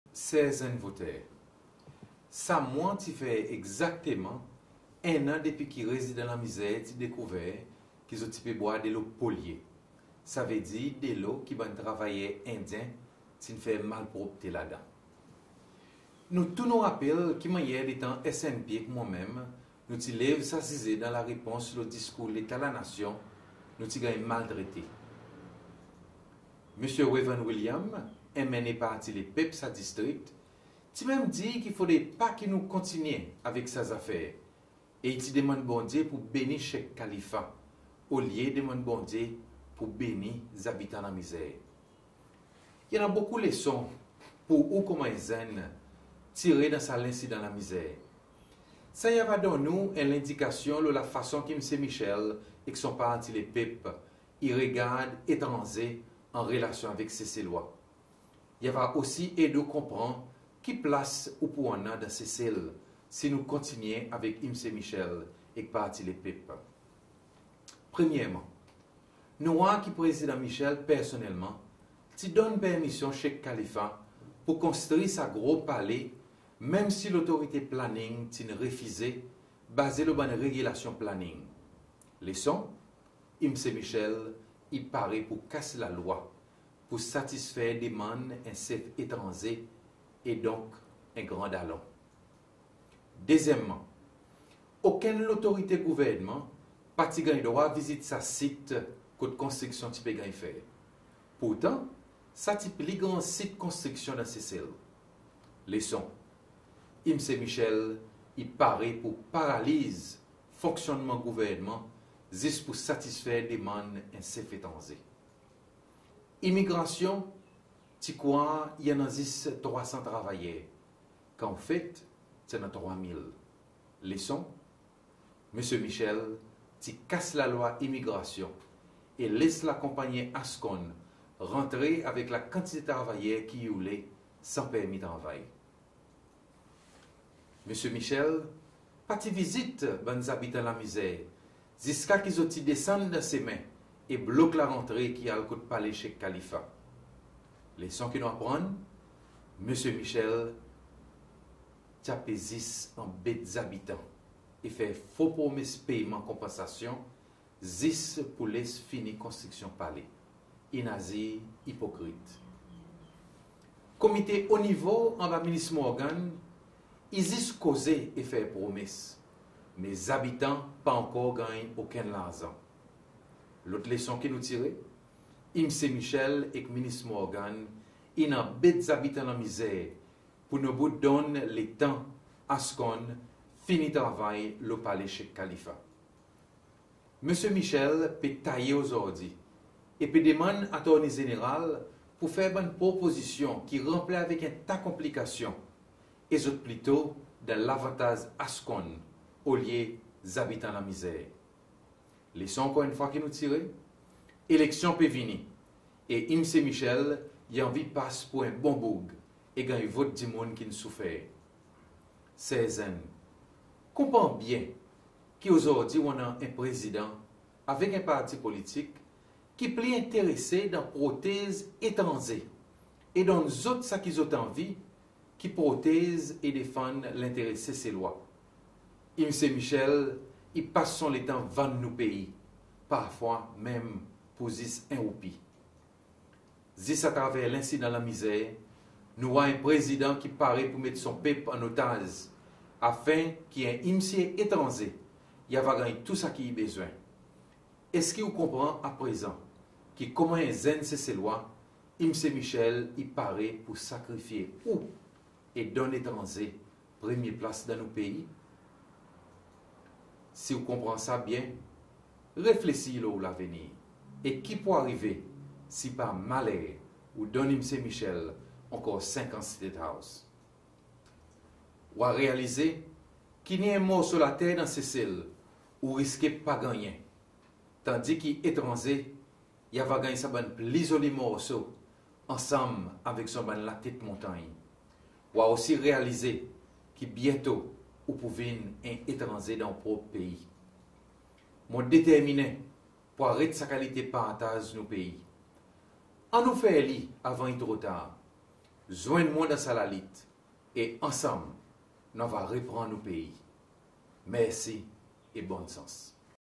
Letting down a community for the world's third richest royal.mp4
To mark the first anniversary of the incident, whereby the La Misere residents had their water supply contaminated by human excrement by workers on the site of Sheikh Khalifa's palace, Mr Wavel Ramkalawan addresses the injustice they suffered. The whole saga is a perfect example of the ruling party's governing style: duplicity, broken promises, contradictory statements and deadlines set back and forth. The incident had a valuable lesson for all. But most of all, it teaches us the need to stand up for one's right.